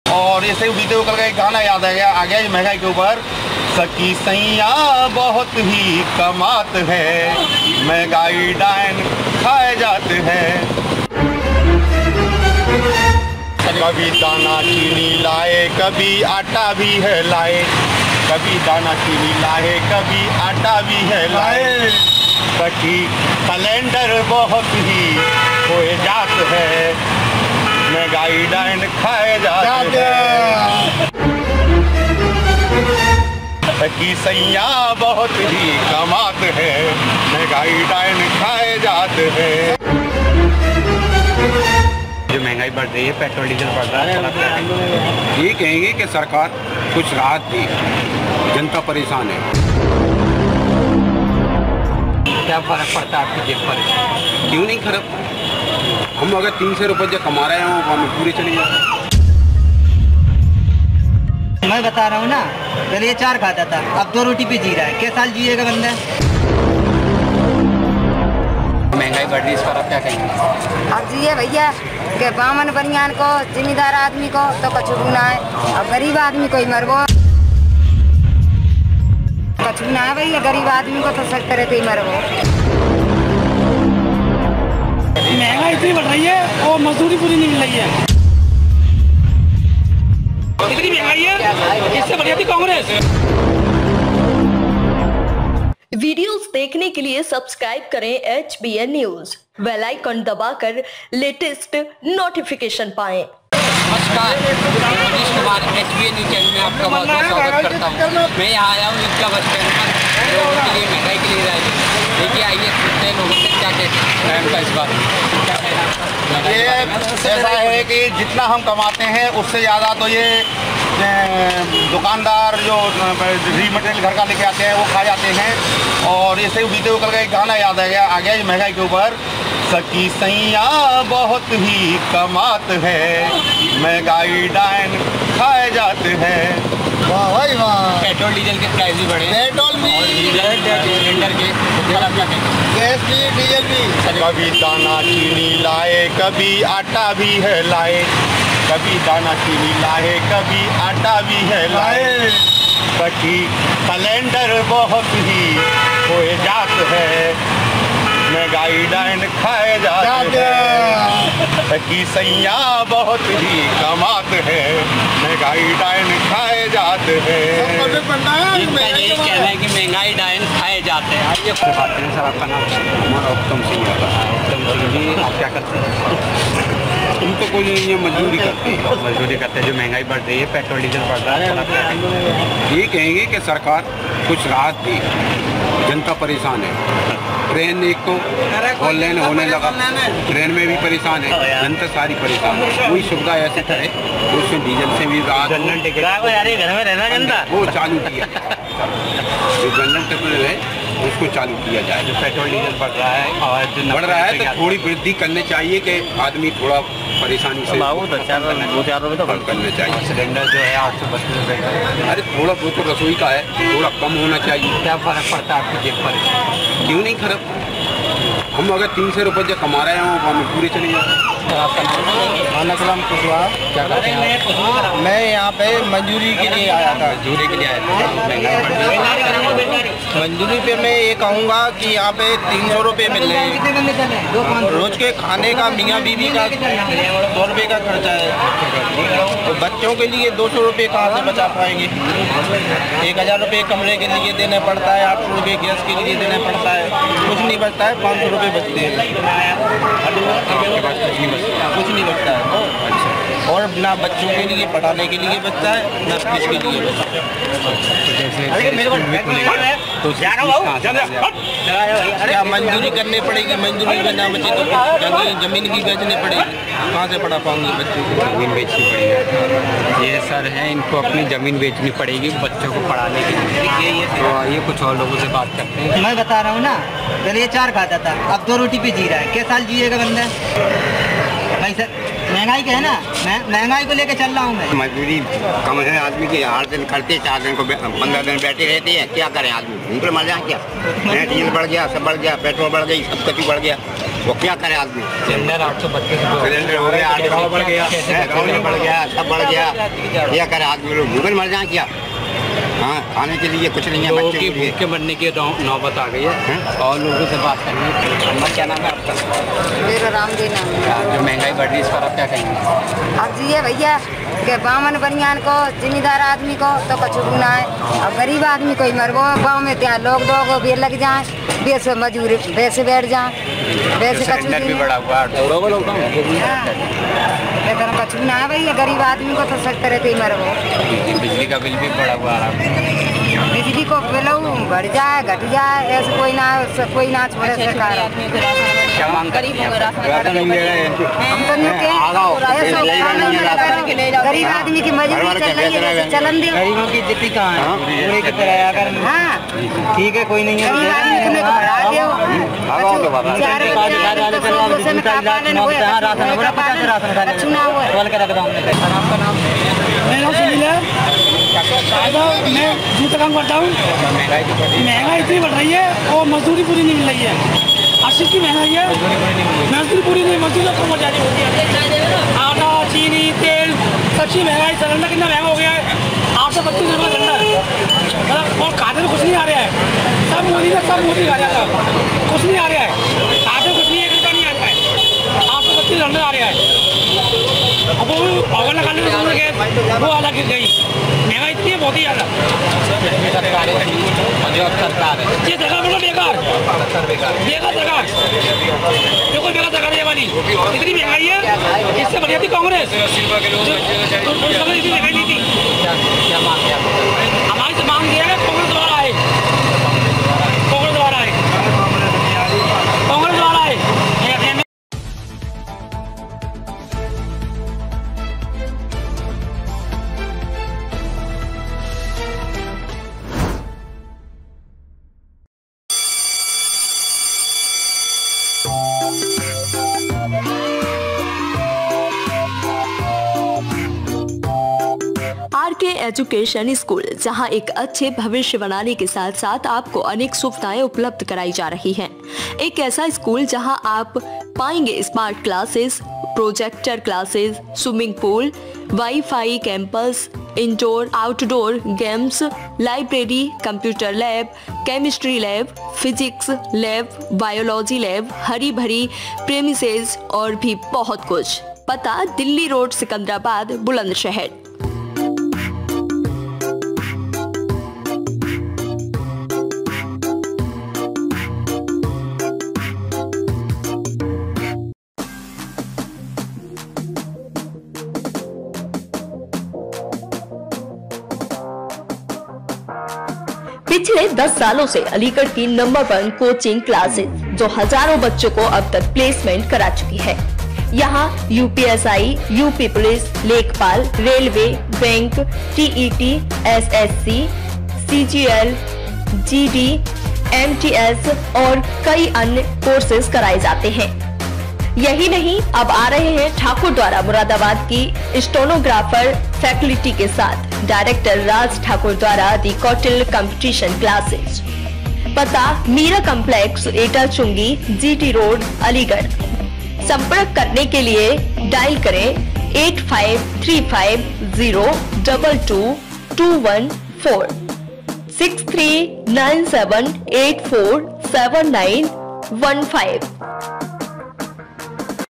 मंहगाई को लेकर आगरा वासियों ने बीजेपी सरकार को बुरी तरह से धोया | HBN News
मंहगाई को लेकर आगरा वासियों ने बीजेपी सरकार को बुरी तरह से धोया